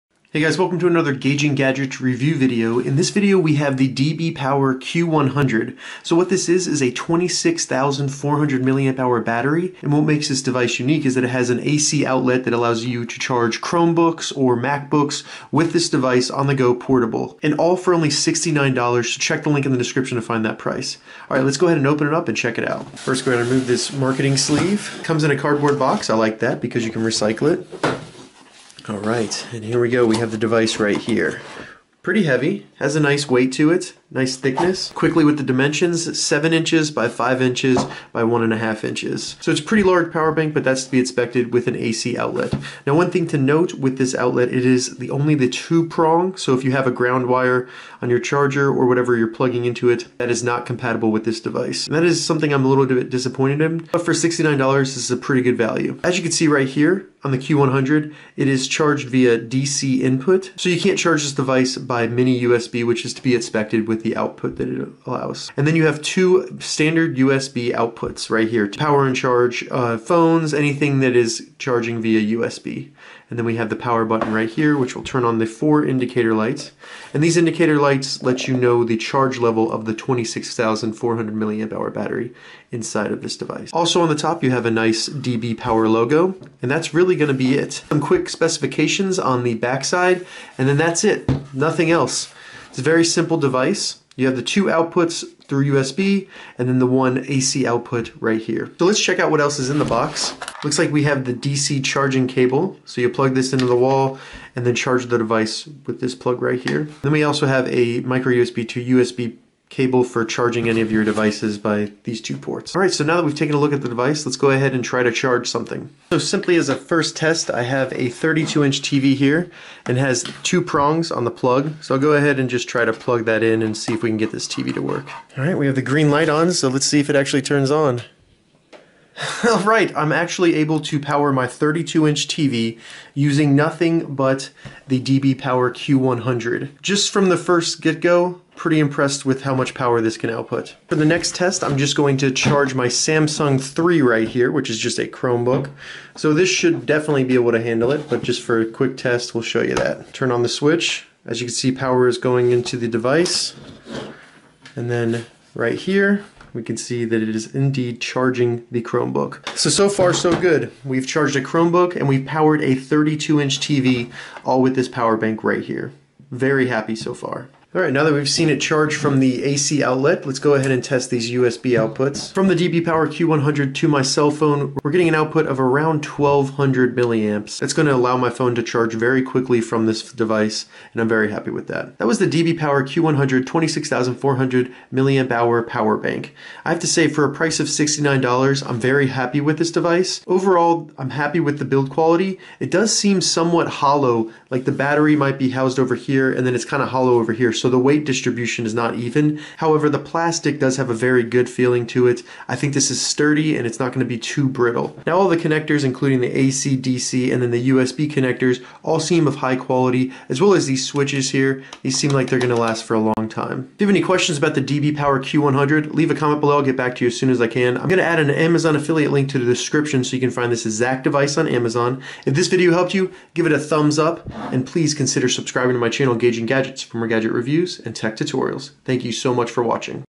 DBPOWER 26400mAh Laptop Power Bank - Q100 AC Outlet Portable Charger
DBPOWER 26400mAh Laptop Power Bank, AC Outlet Portable Power Bank Travel Charger 80W output, Universal Battery Pack for Macbook, Laptops, Smartphones

Looking for a power bank that will allow you to plug in AC powered devices? In this unboxing and review we look at the DBPOWER Q100 Laptop Power Bank. This laptop power bank includes an AC outlet so you can down all of your 80W/1A 2-prong AC gadgets. Additional the DBPOWER power bank features a large 26400mAh portable battery, 2 USB output connectors for charging your phones or tablets, and it recharges in ~2 hours.

Pros of the DBPOWER 26400mAh Laptop Power Bank:
 -  Low cost compared to similar power banks
 -  Ability to simultaneously charge two USB devices and the AC outlet at the same time
 -  4 very bright LED indicators
 -  Build quality is high. This device seems to be well made and sturdy.  -  The weight distribution is not balanced at all with most of the weight being towards the front.
 -  Similar to other AC Power Banks from brands like Anker, RAVPower, NexGadget, OMARS, Jackery, Omnicharge, MAXOAK, VINSIC, imuto, ChargeTech
 -  Through my testing I found that the power bank was able to power several different devices such as a 32 inch TV, oscillating fan, and lights. The charge last for several hours with no issues.

Cons of DBPOWER 26400mAh Laptop Power Bank:
 -  At 7.2 x 4.8 x 1.6-inch it 's much larger than other power banks of similar capacity
 -  Requires AC charging. It is not possible to charge this device with a USB charger
 -  AC outlet only compatible with 2-prong plugs

My YouTube Setup:
Mpow PC Headset
Cell Phone - Pixel 2 XL